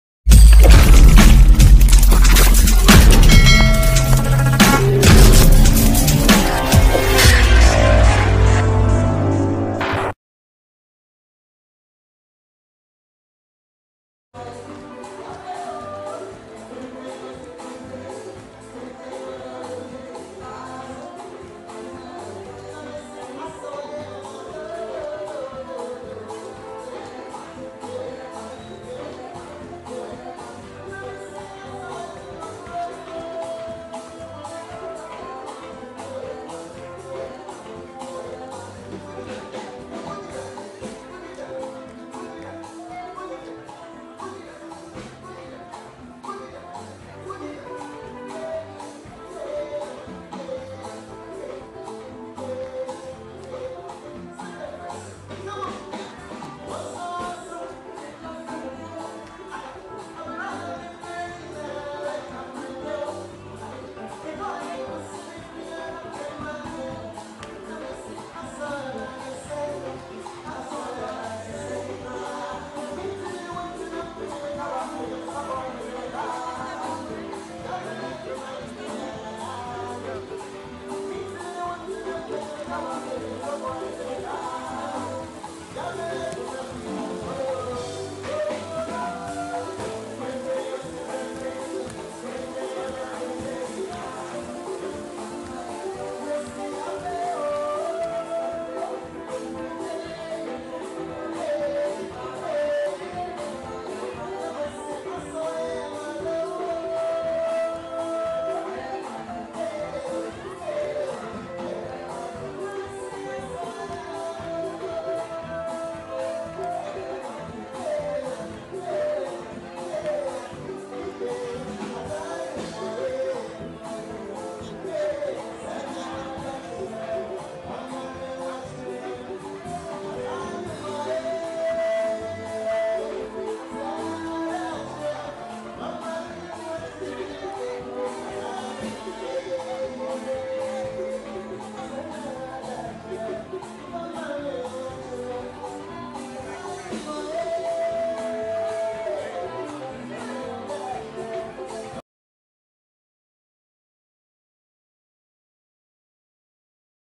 President Akufo- Addo And Wife Display Killer Dancing Moves At Their 20th Wedding Anniversary
Ghanaian Watch Tv gives credit Ghana Web Tv as the original source of the video.